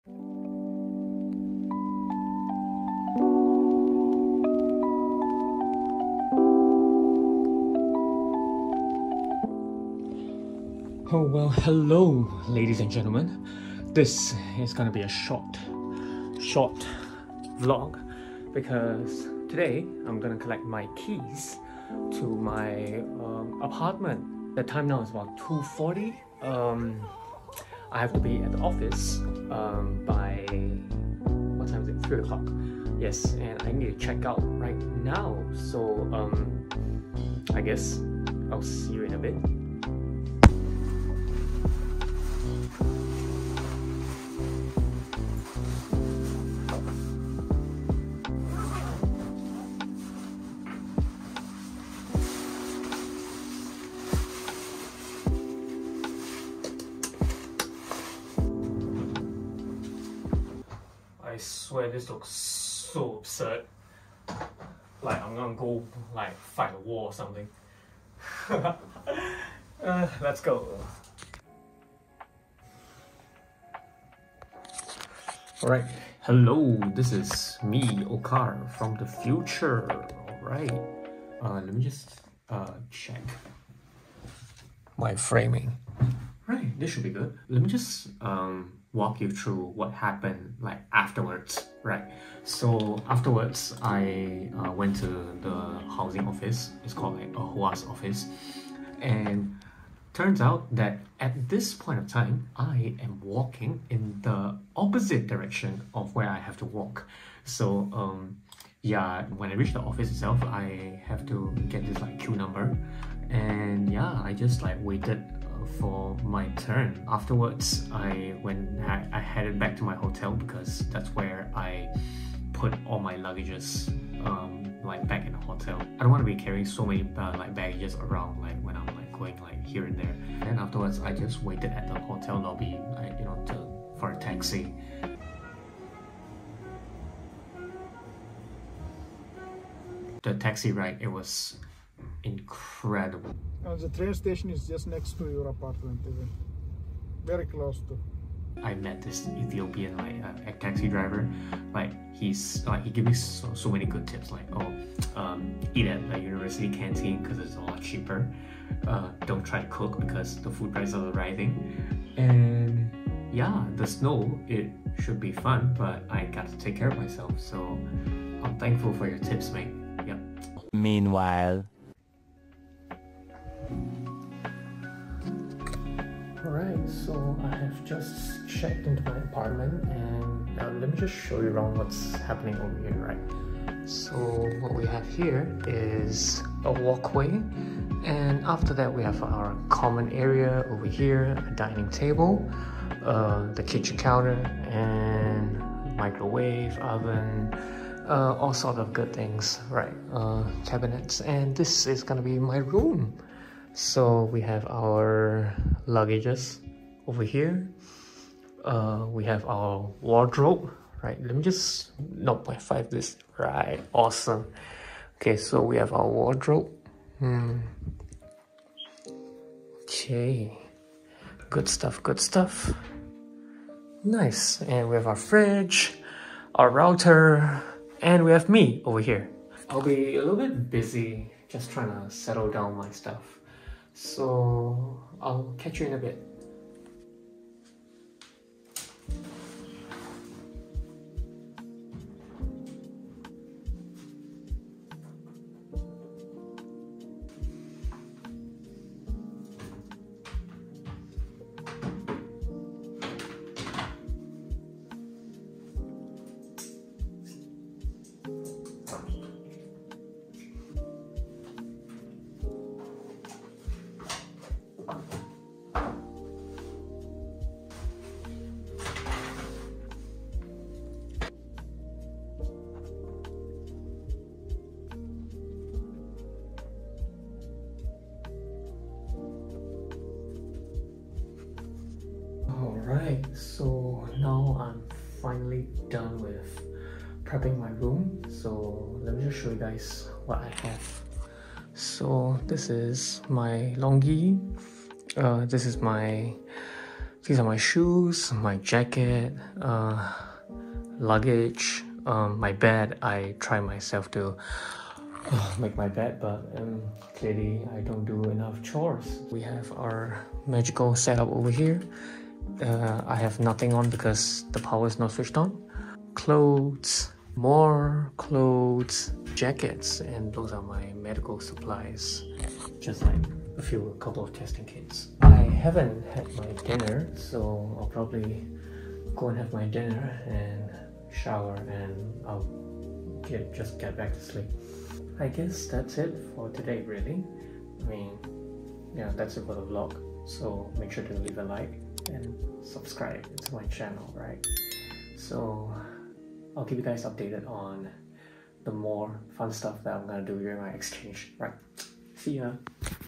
Moving into my apartment in a beanie (Episode 2: Oakar's Finnish Adventures 🇫🇮 )
Just a short vlog of me moving into my apartment.
First time filming all footage in 60fps and rendered using 60fps so hopefully this video is a lot more smoother to watch.

Lastly, you know what to do.